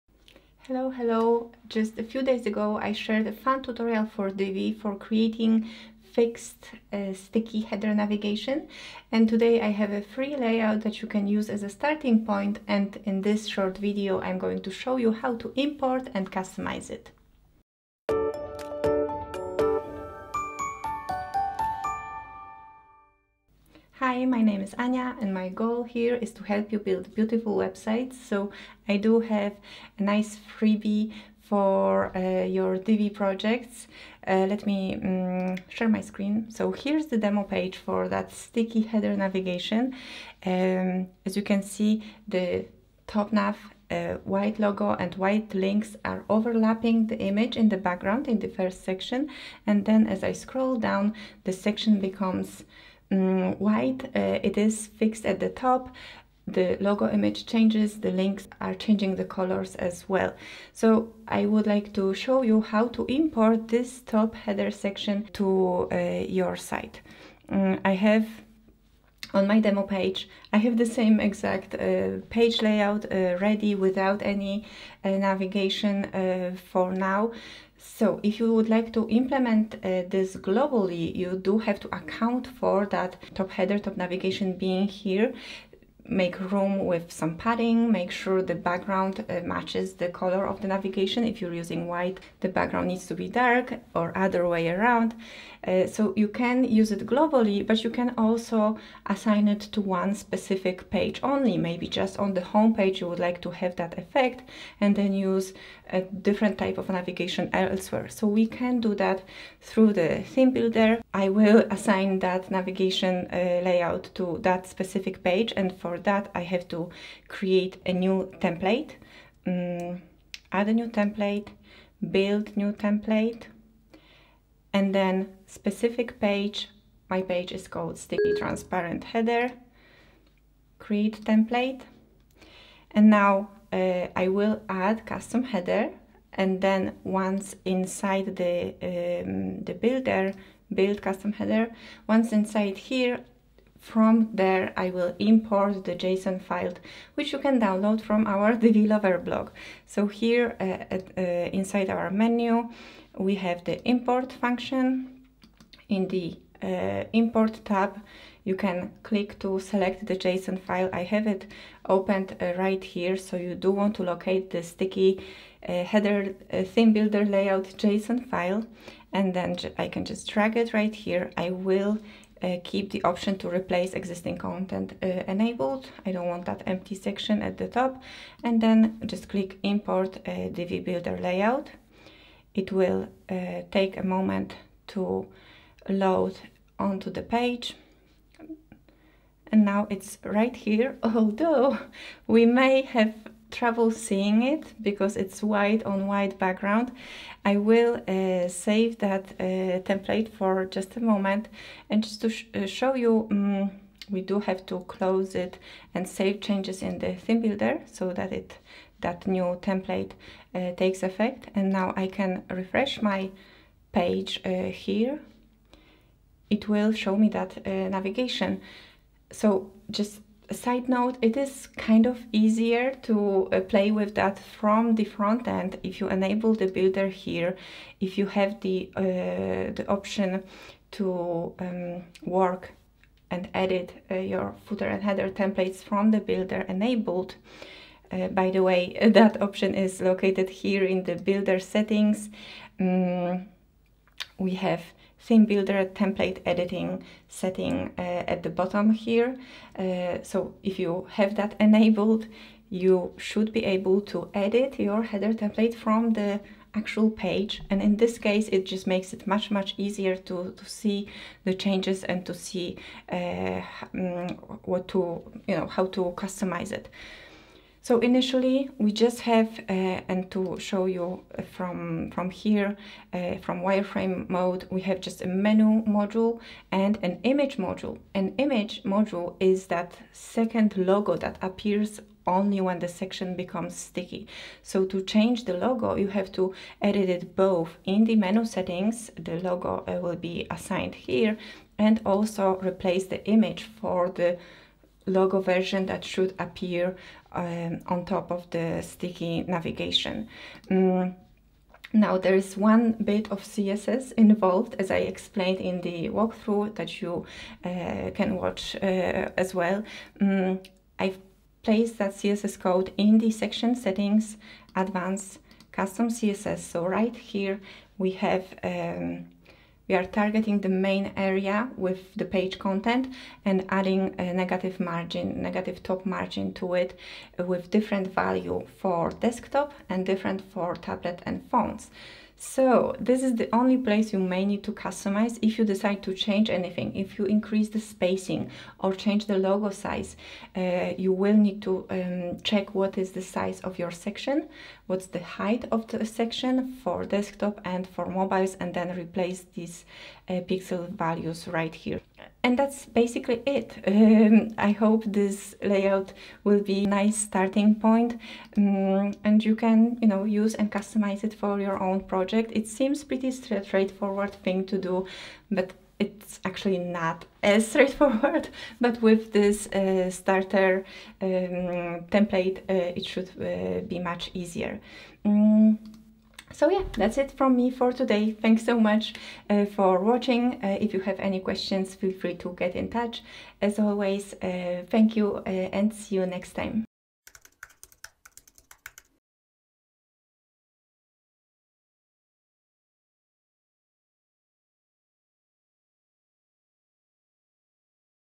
Free Transparent Sticky Header Layout for Divi ~ Download & Customize
Get ready to upgrade your Divi website with a free transparent fixed header layout! In this video, I’ll show you how to import, and customize the layout to fit your brand’s style.

This layout is perfect for giving your site a sleek, professional look with minimal effort. Plus, I’ll share the simple CSS snippet used to create this effect. Interested in taking your Divi skills further or working with me directly?

~ Ania R.